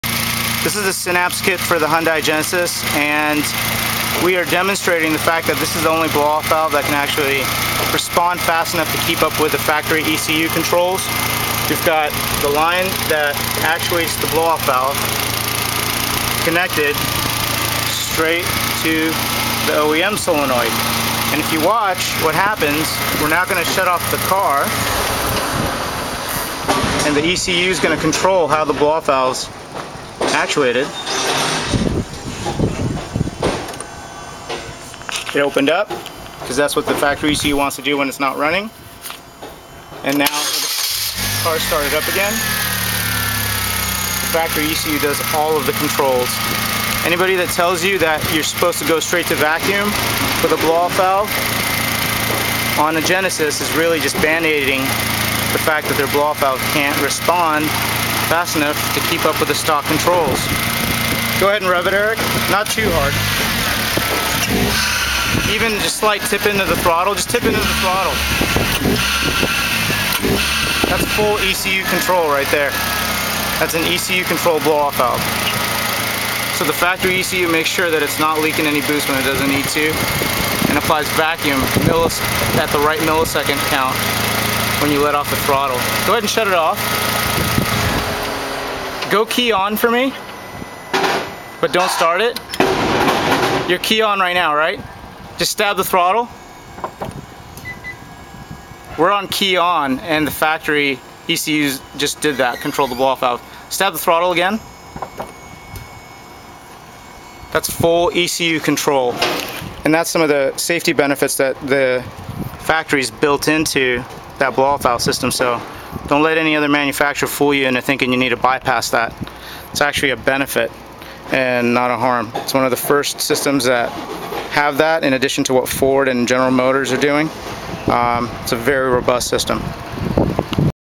Synapse BOV Hyundai Genesis
Peter is demonstrating how the Synapse Engineering Blow Off Valve can operate with the factory ecu for the stock Hyundai Genesis. This blow off valve works off the factory solenoid because it can react fast enough to keep up with how fast the ecu wants it to operate.